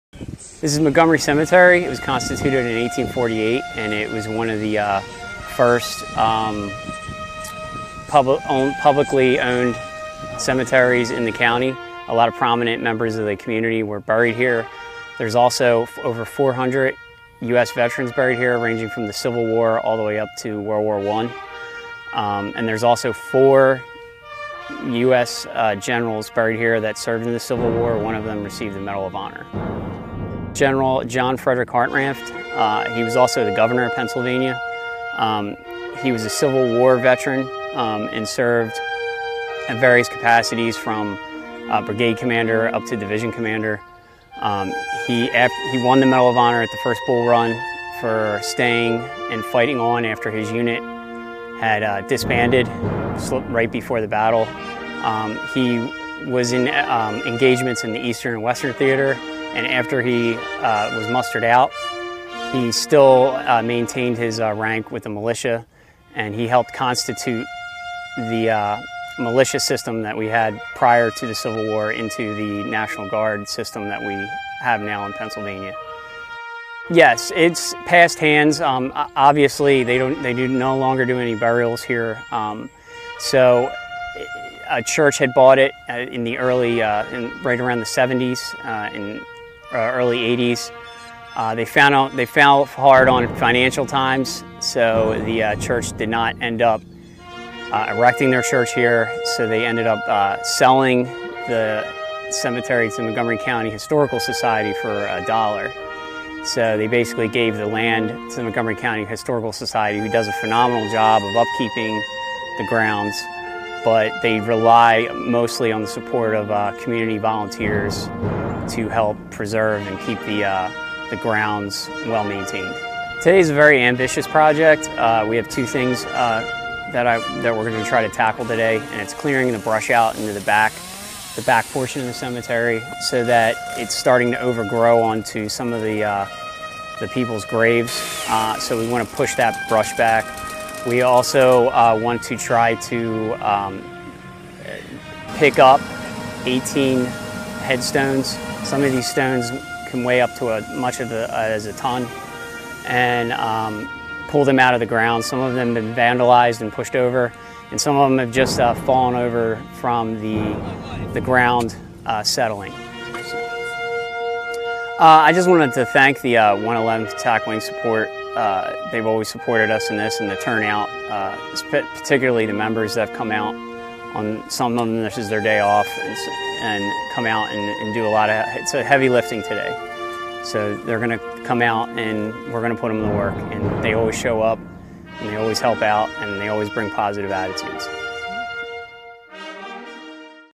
111th Members volunteering at Historic Montgomery Cemetery to pay respect to history
111th Members volunteering at Historic Montgomery Cemetery to pay respect to history
HORSHAM, PA, UNITED STATES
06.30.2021

Master Sgt. Andrew Oreo, 111th Logistics Readiness Squadron and 111th Civil Engineering Squadron First Sergeant, explains why volunteering at Historic Montgomery Cemetery in Norristown, Pennsylvania, is important to him and members of the 111th Attack Wing. (U.S. Air National Guard video by Senior Airman Wilfredo Acosta)

Film Credits: Video by Senior Airman Wilfredo Acosta
111th Attack Wing

 Pa Air National Guard
Pennsylvania Air National Guard
Biddle Air National Guard Base
111th Attack Wing
 Historic Montgomery Cemetery